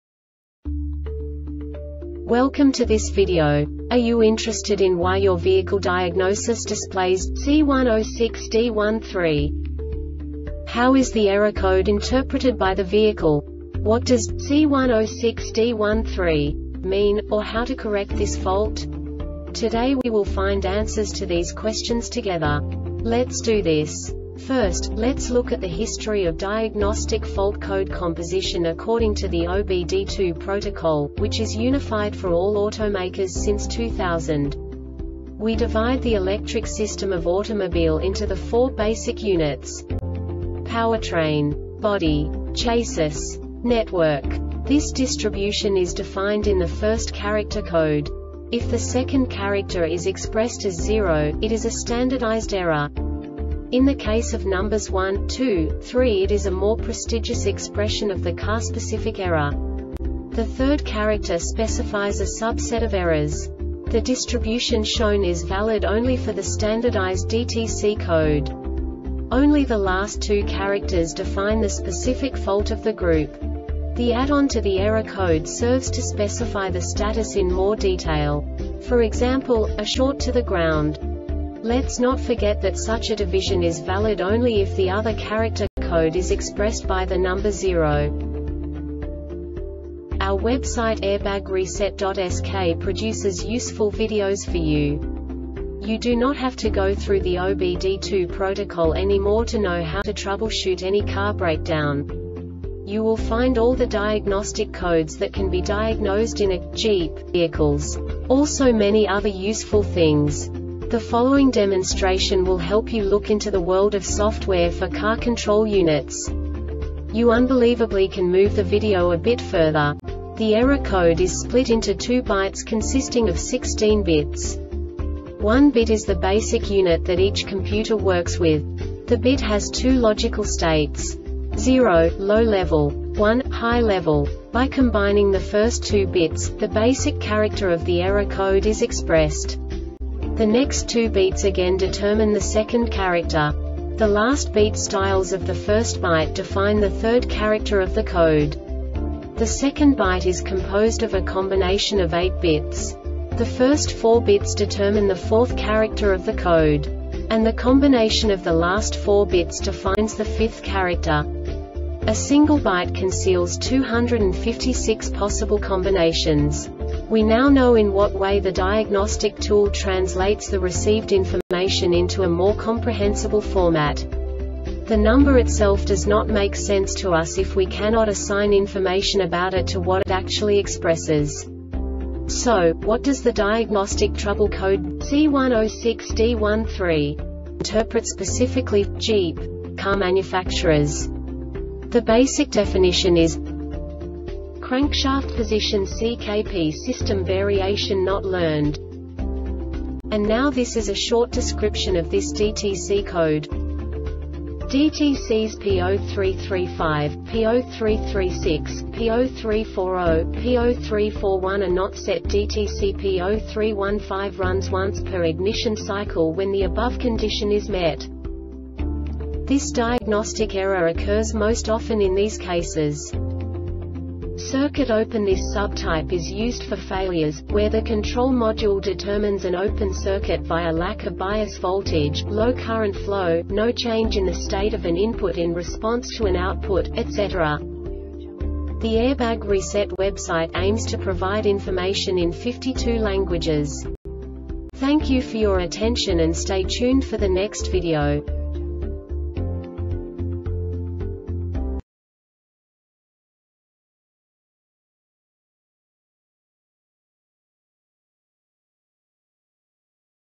DTC Jeep C106D-13 Short Explanation
Contents:
0:21 Basic DTC analysis according to OBD2 protocol standard.
1:48 Insight into programming
2:58 A diagnostically understandable description of the Diagnostic Trouble Code
3:05 Basic error definition

Make:
Jeep
Code:
C106D 13
Definition:
Crankshaft Position (CKP) System Variation Not Learned
Description:
DTCs P0335, P0336, P0340, P0341 are not set.DTC P0315 runs once per ignition cycle when the above condition is met.
Failure Type:
Circuit Open